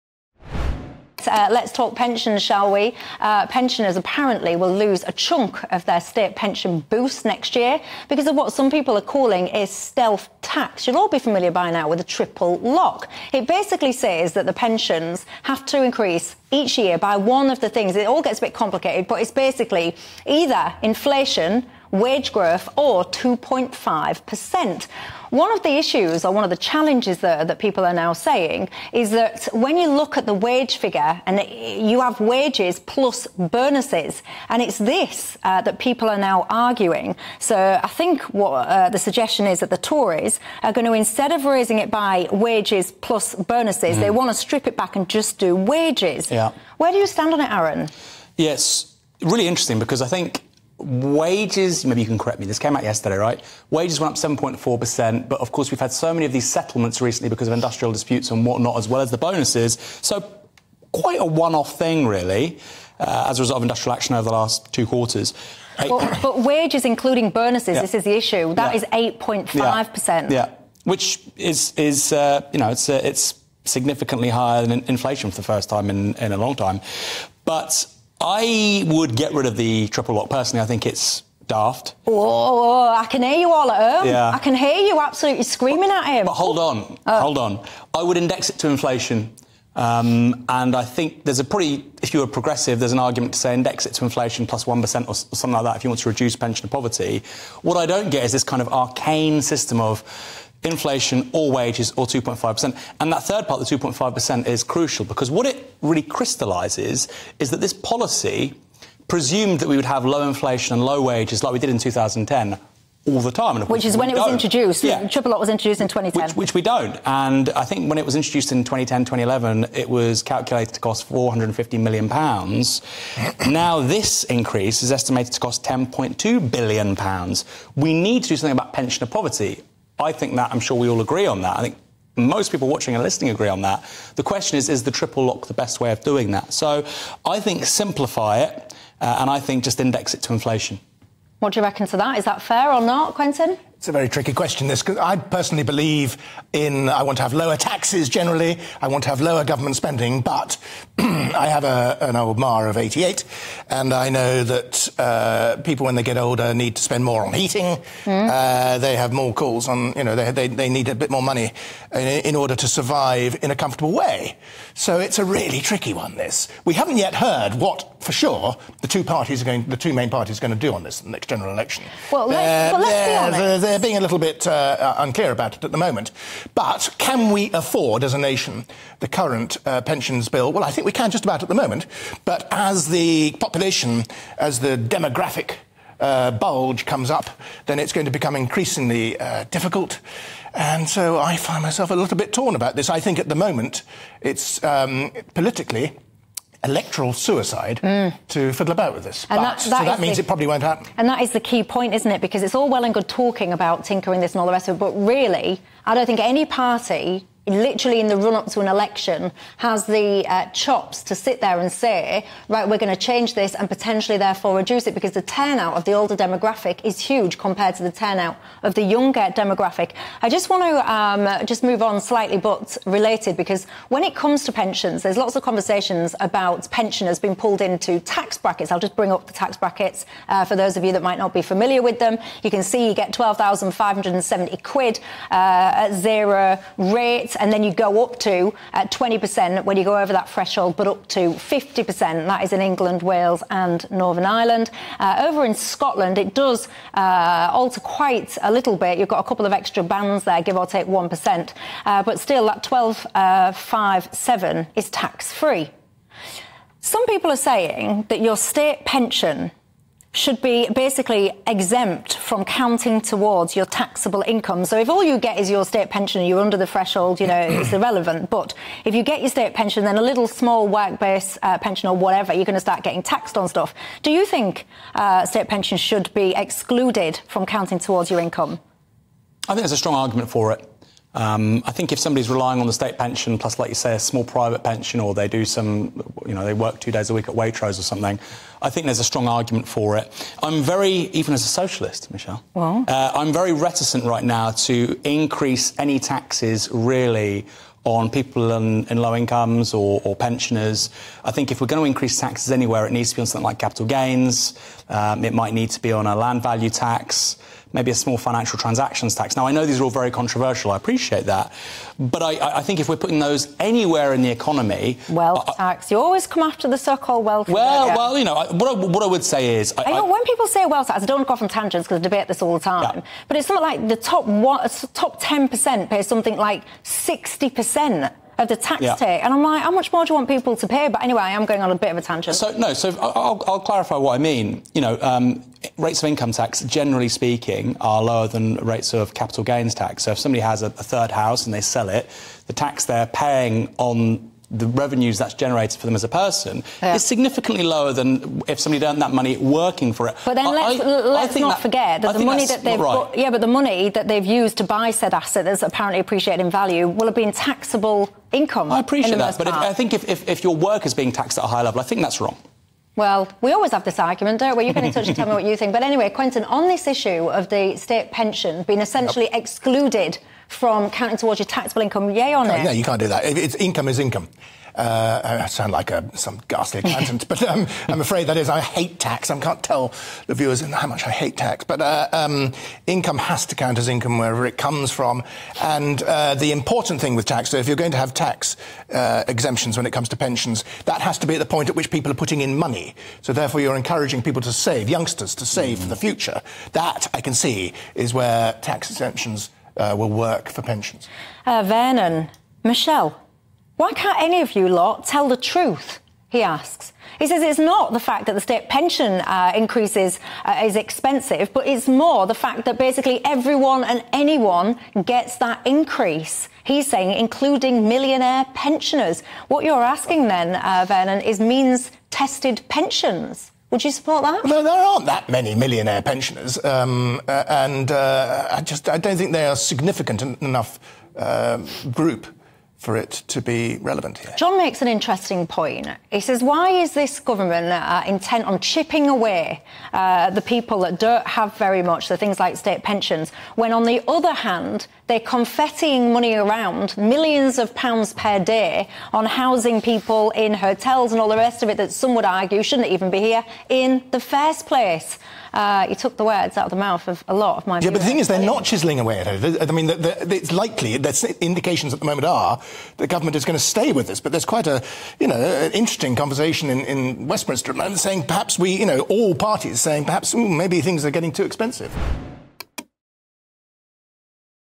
'Pension triple lock is daft': Should we keep it? | Aaron Bastani and Quentin Letts
'I would get rid of the triple lock, I think it is daft.'

Co-founder, Navara Media, Aaron Bastani, debates with Journalist, Quentin Letts, over whether we should keep the triple lock pension.